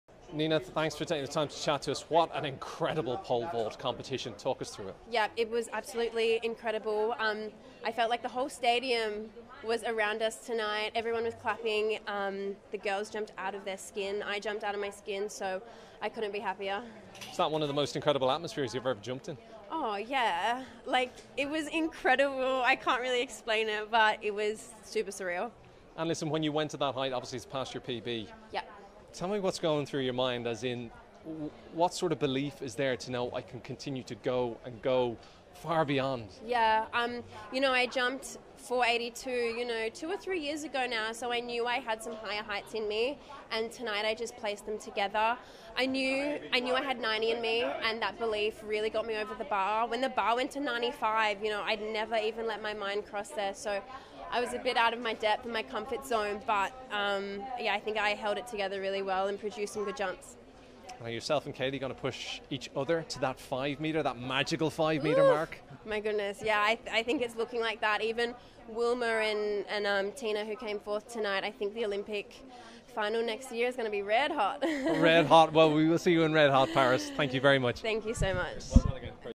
Nina Kennedy on sharing the Pole Vault Gold Medal with Katie Moon!
"It was incredible, I can't really explain it, surreal!"

Nina Kennedy after winning gold in the Women's Pole Vault Final alongside Katie Moon after agreeing to share the medal. 🇦🇺🤝🇺🇸 🥇

Watch the 2023 World Athletics Championships live on Virgin Media Two.